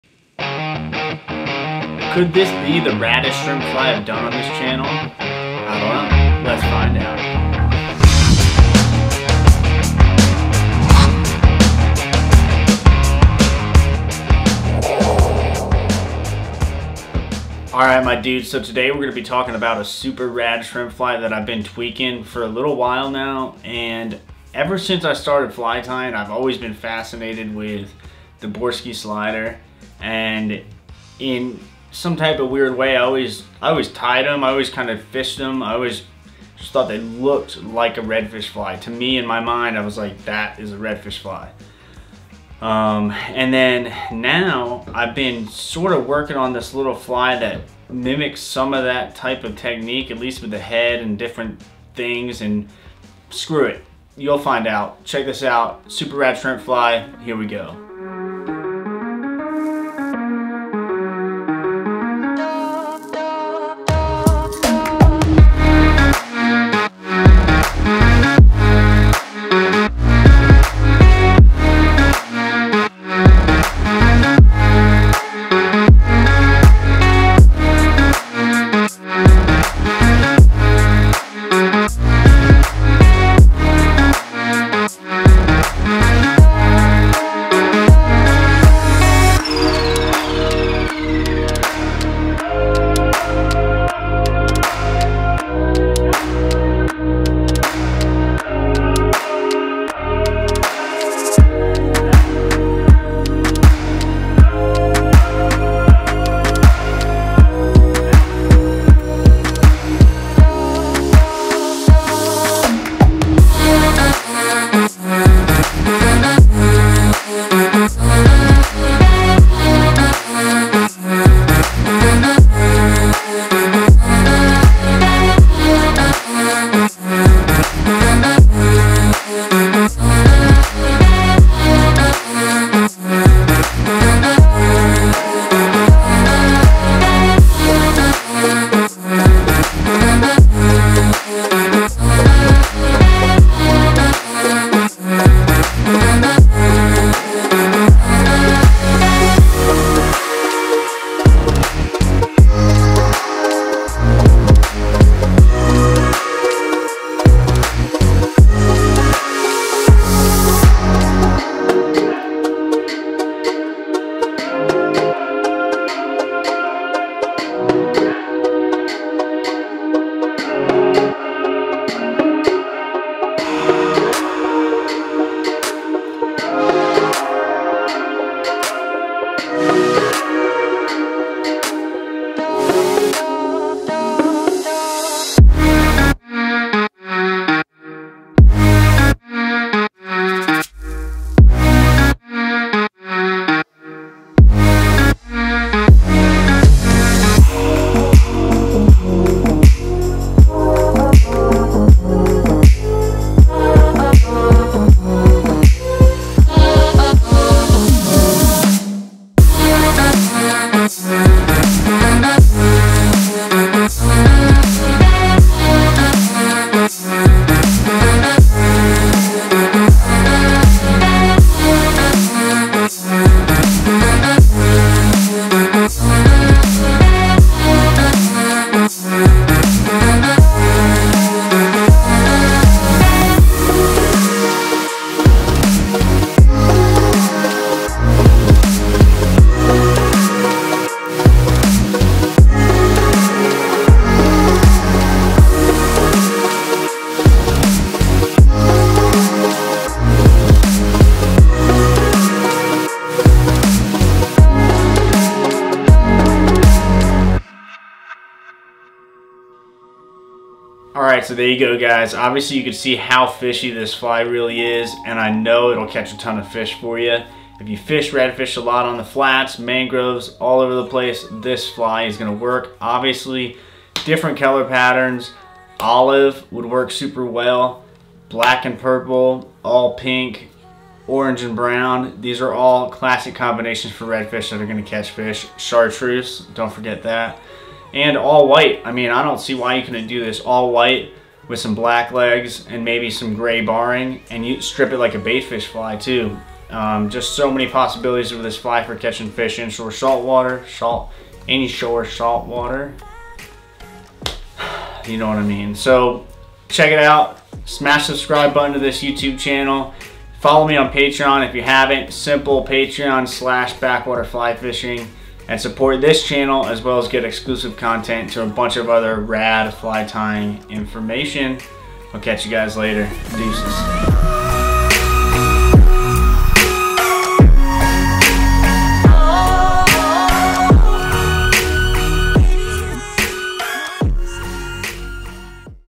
How To Tie A Deer Hair Redfish Slider Fly: BACKWATER VLOG #65
I am super stoked to show you guys this Deer Hair Redfish Slider as it is one that I have been holding back on for a while now just because for some reason I have always been modifying it and always felt like it was missing something...well...those days are over and I think I finally have this redfish slider dialed in.